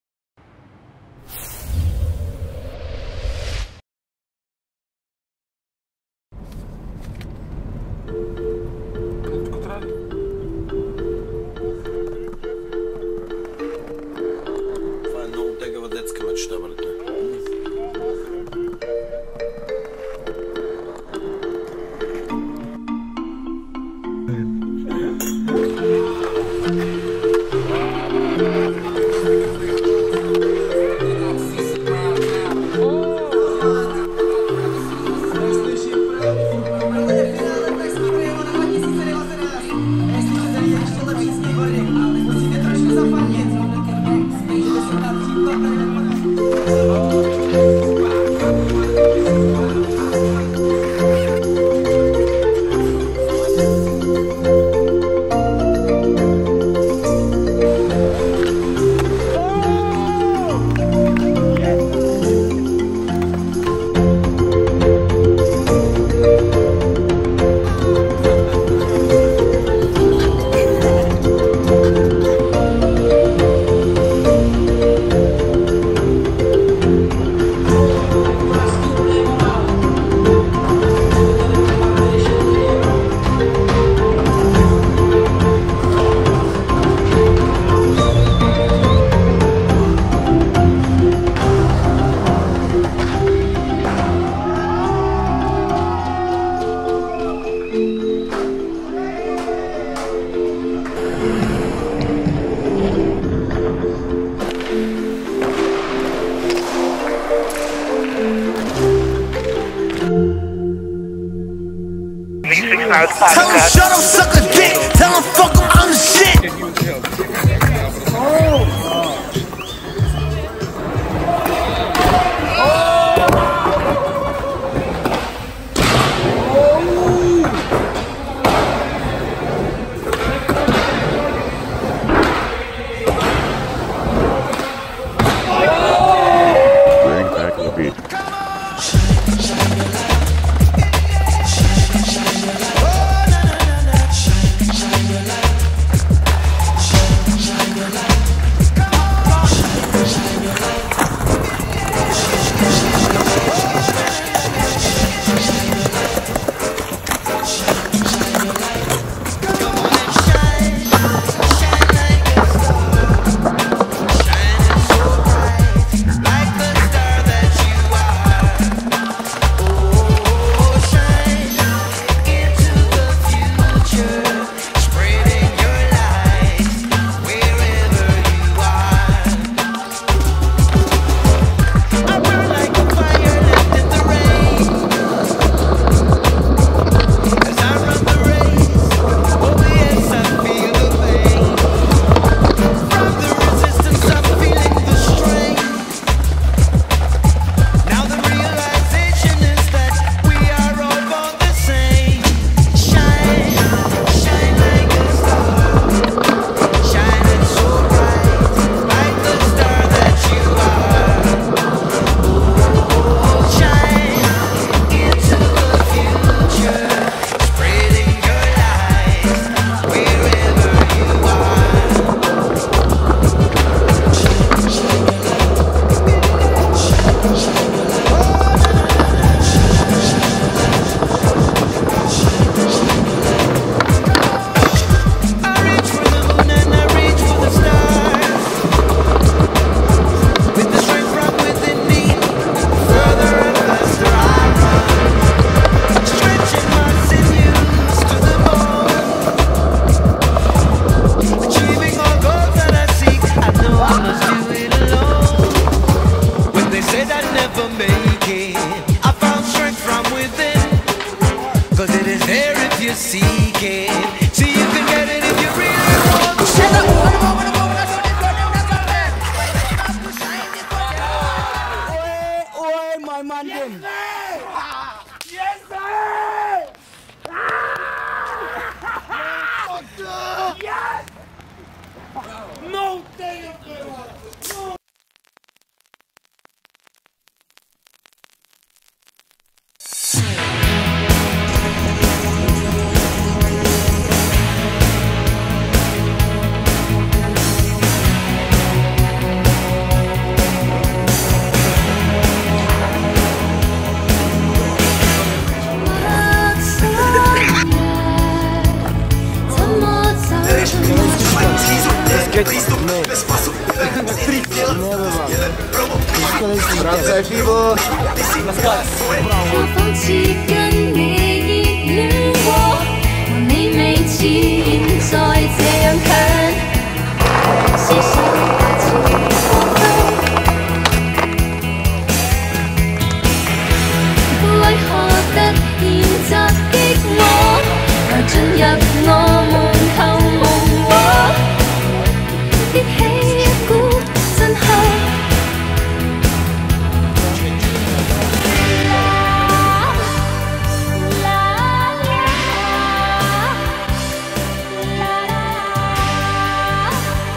BLOK BOYS Killing & Chilling Vol. 14 - Prague Trip 2019
В този епизод BLOK BOYS се отправят към Прага. 3 дни, 3000 км, Mystic Park, Stalin Plaza, Bowl Bar! Благодарности на партньорите ни от Ambassadors за незабравимите моменти! 9 дни след тези кадри, на Stalin Plaza карането е забранено.

SAVE STALIN PLAZA!

BLOK BOYS

Riders:

Marti Atanasov - IG: @martin_sk8atnsv
Kristian Evtimov - IG: @keenanblokboy4life
Nikolay Elenkov - IG: @n.i.k.i.e.l.e.n.k.o.v.
Yan Lozov - IG: @skiboder
Veliko Balabanov - IG: @veliko.balabanov

Produced by BLOK Supply Co.
Filmed by Veliko Balabanov
Edited by Nikolay Petrov